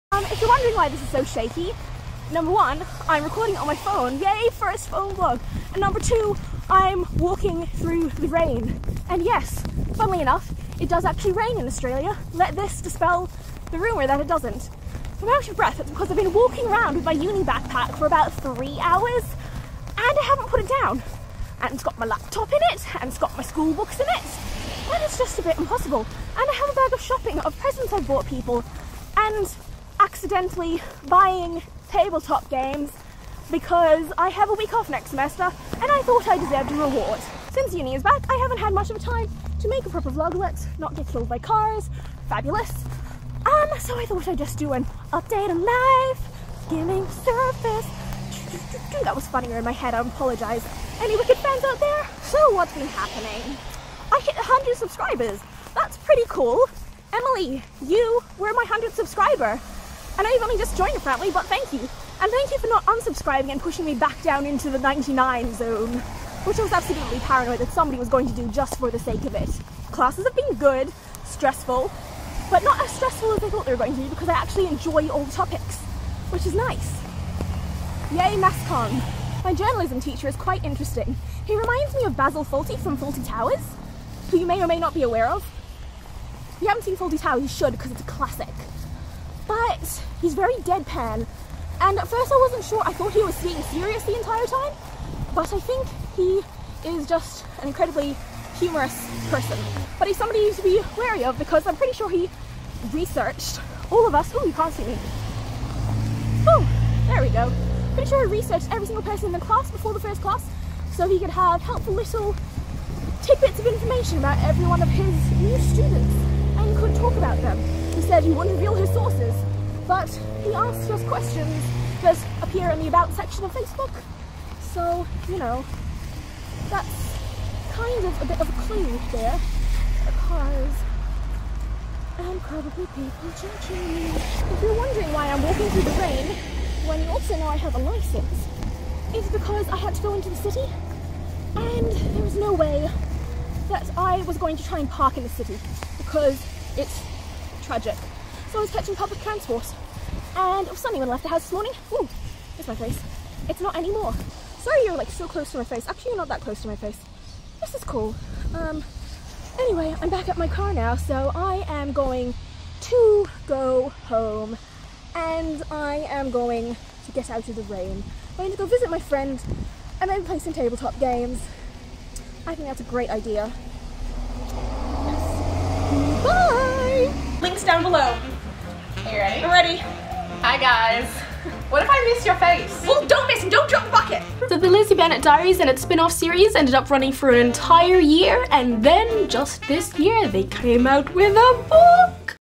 Rainy Day Update
Have a chat with me while I stroll through the rain.
I discovered while I was filming this that my boots are not nearly as waterproof as I thought they were and my heart broke a little.

Congratulations, you read my description! You get a little gold star for being super cool.